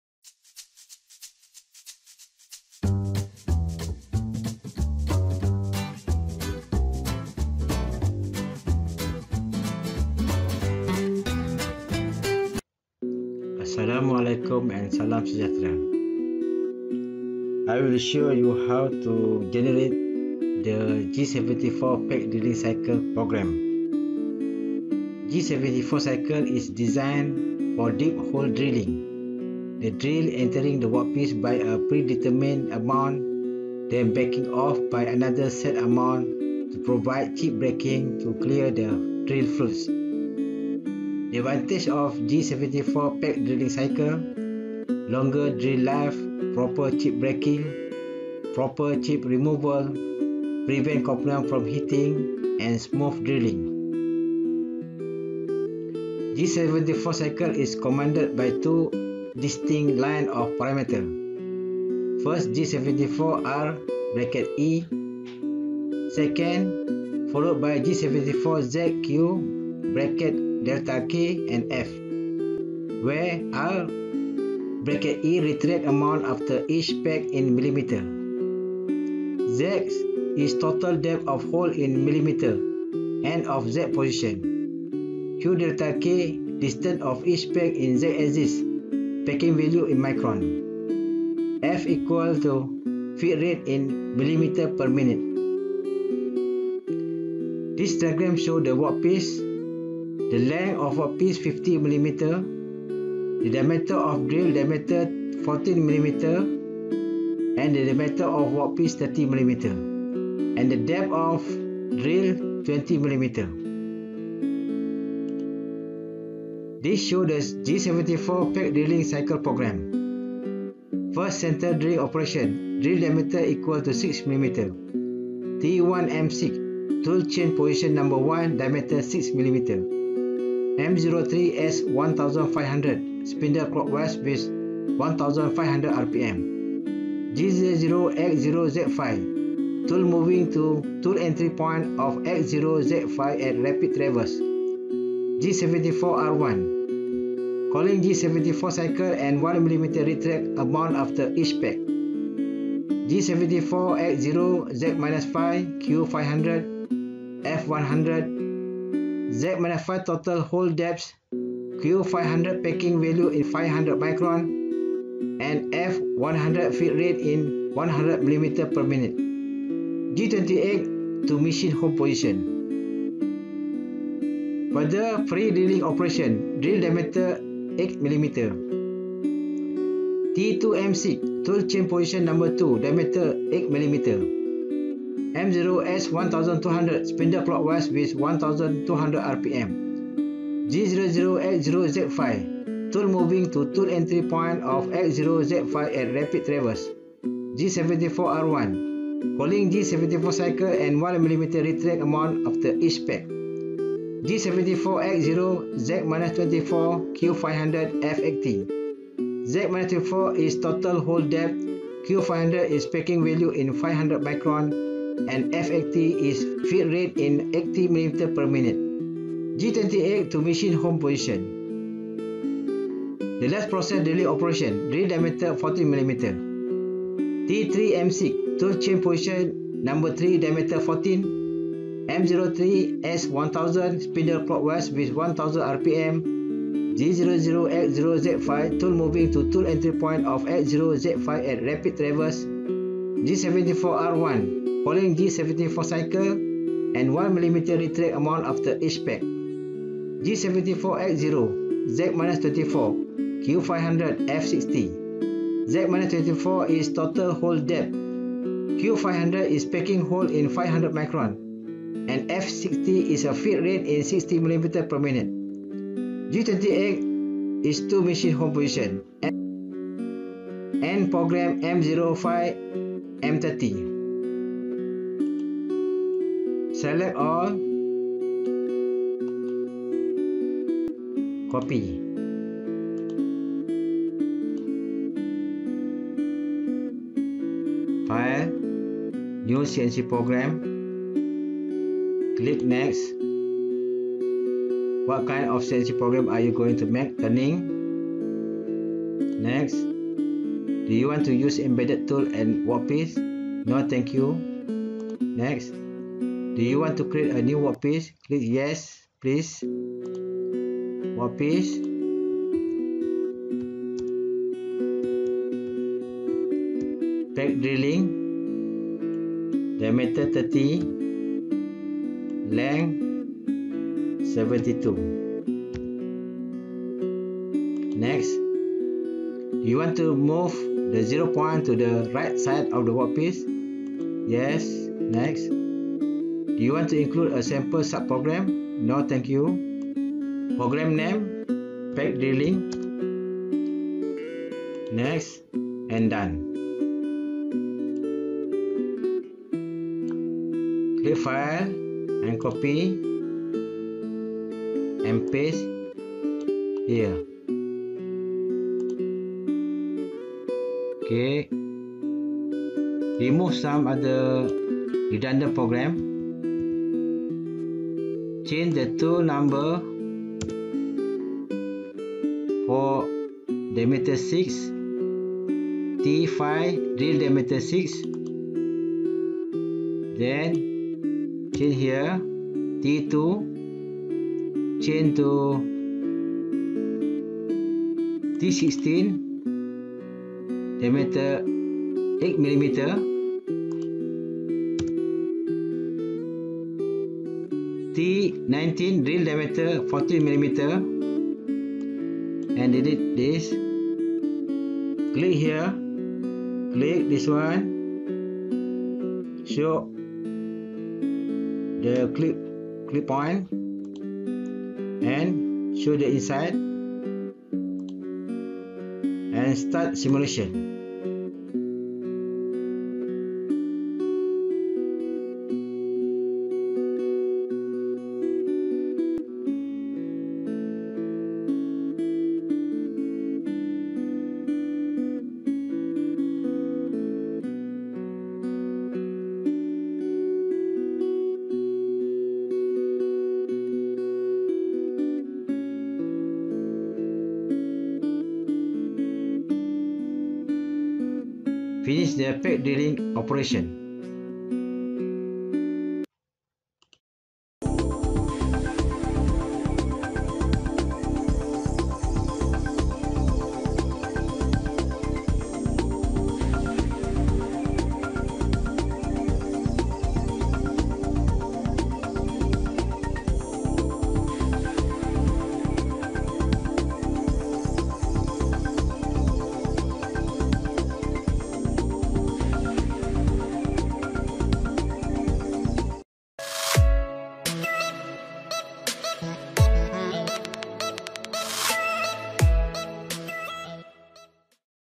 G74 PECK DRILLING CYCLE USING CNC SIMULATOR PRO
G74 cycle is commanded by two distinct lines of parameters
• G74 R (e)
• G74 Z Q (delta k) F
Where:
R(e) = Retract amount after each peck in mm
Z = Total depth of hole in mm (end of Z position)
Q (delta k) = Distance of each peck in Z axis (Pecking value in micron)
F = feed rate in mm/min